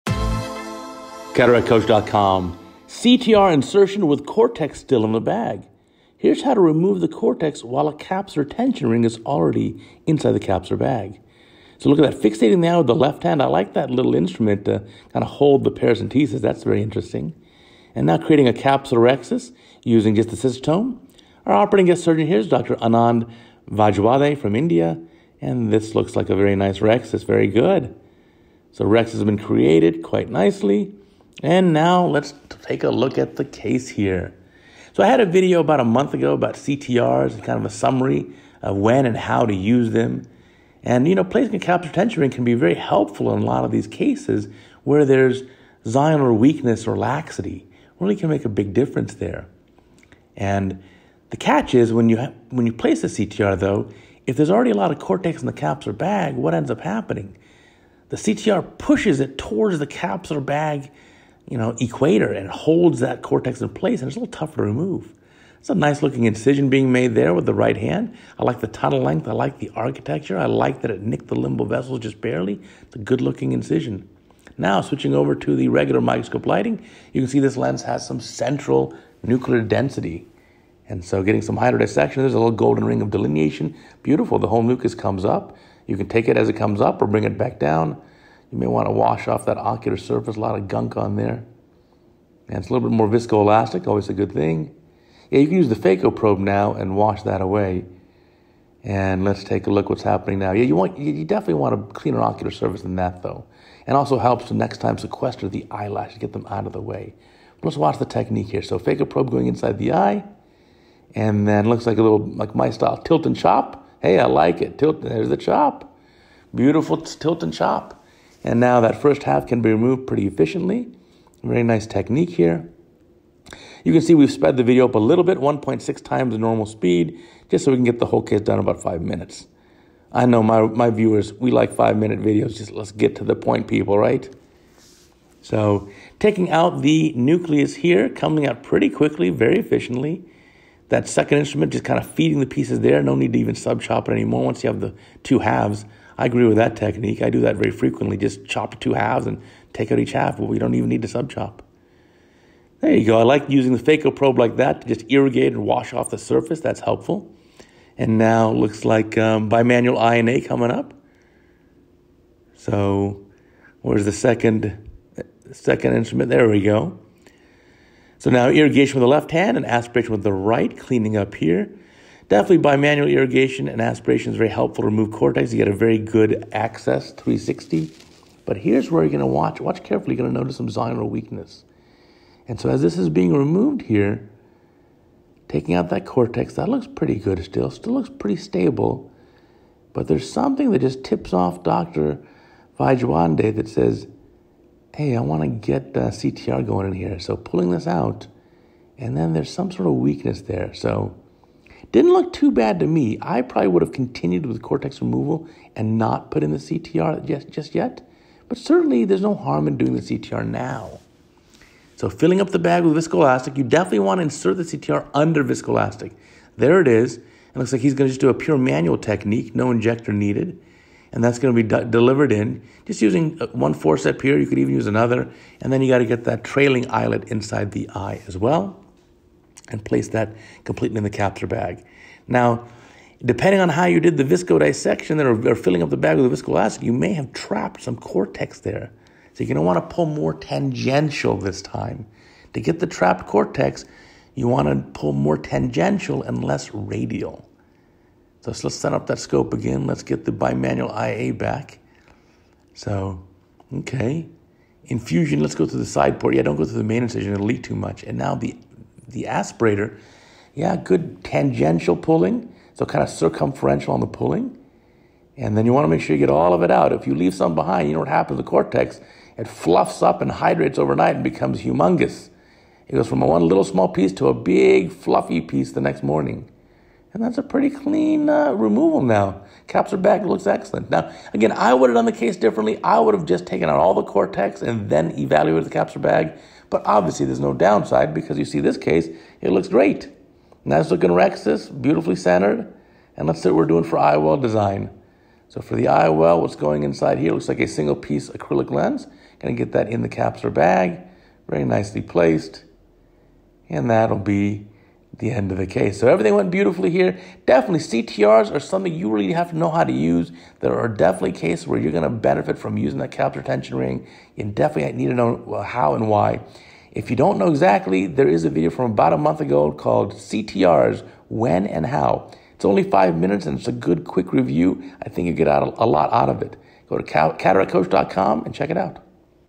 CataractCoach 1390: CTR insertion with cortex still in bag
It is easiest to insert the capsular tension ring (CTR) when the capsular bag is completely empty of lens material. If we insert the CTR while there is still cortex present, the CTR can trap the cortex at the capsular bag equator which makes removal challenging. In this case the surgeon felt the need to insert the CTR prior to removing all of the cortex and he shows the approach to successfully overcome this challenge. As the saying goes, insert the CTR as early as you need it, but as late as you can.